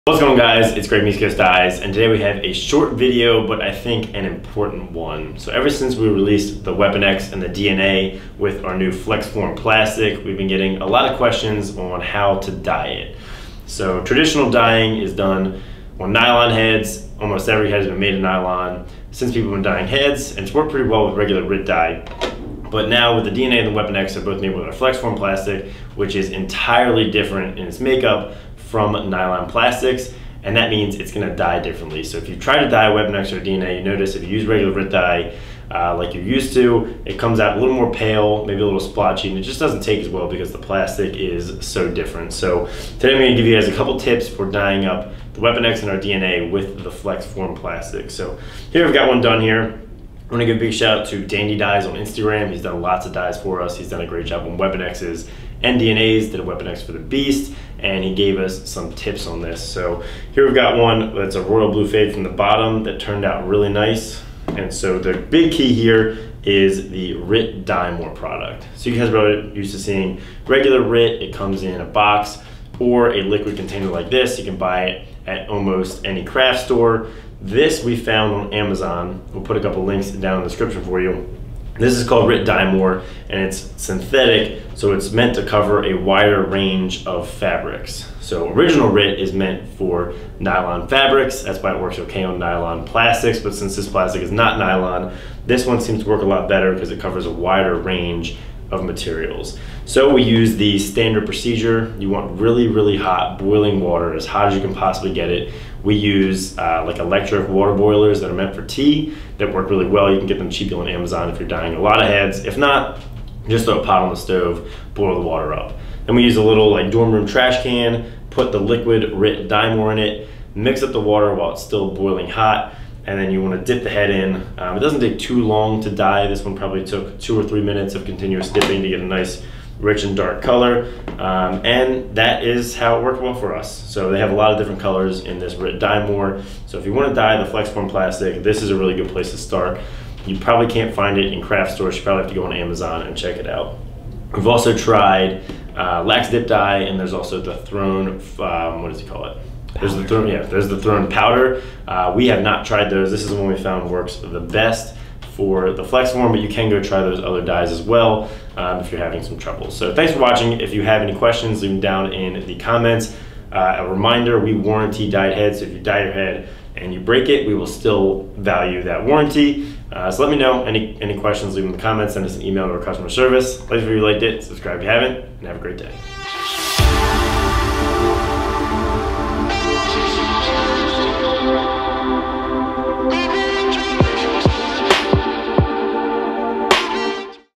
How to dye FlexForm Plastic (DNA & Weapon X)
ECD Greg shows you how to dye our FlexForm plastic used in our DNA and Weapon X heads.

Rit Dye More: